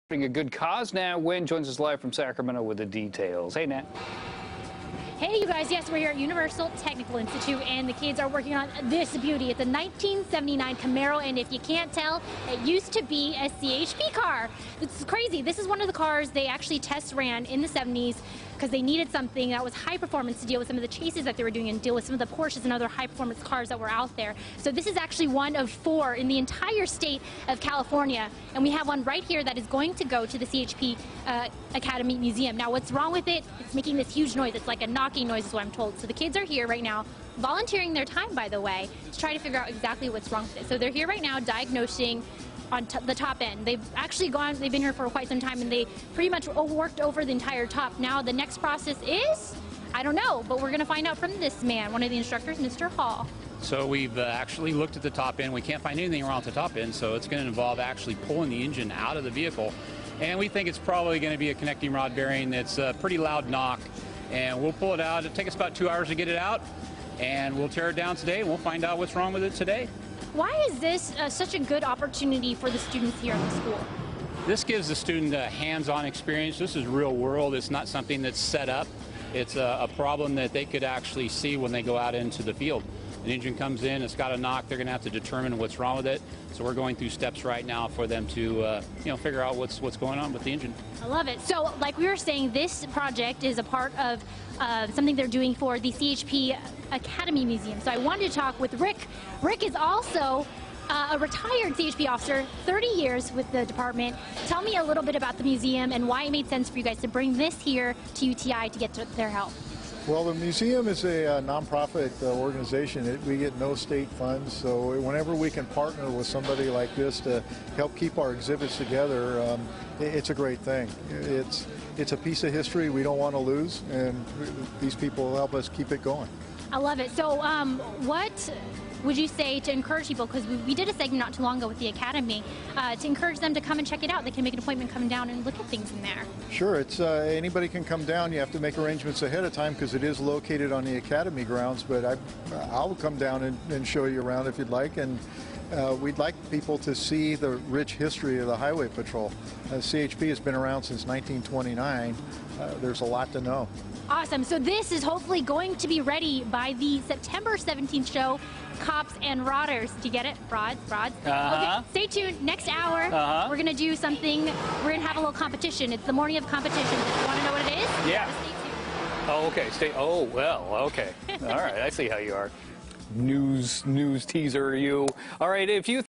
UTI Rebuilds Cars
Students from the Universal Technical Institute are putting their education to the test for a good cause.